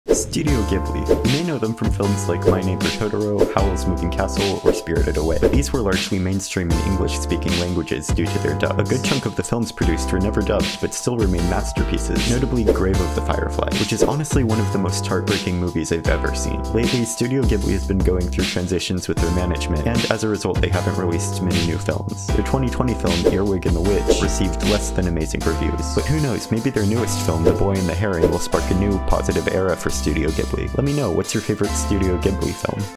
studio ghibli
script:
You may know them from films like my neighbor totoro, howl’s moving castle, or spirited away, but these were largely mainstream in english speaking languages due to their dubs. A good chunk of the films produced were never dubbed, but still remain masterpieces, notably grave of the fireflies, which is honestly one of the most heartbreaking movies i’ve ever seen. Lately, studio ghibli has been going through transitions with their management, and as a result, they haven’t released many new films. Their 2020 film, earwig and the witch, received less than amazing reviews. But who knows, maybe their newest film, the boy and the heron, will spark a new positive era for studio ghibli. Let me know, what’s your favorite film by them?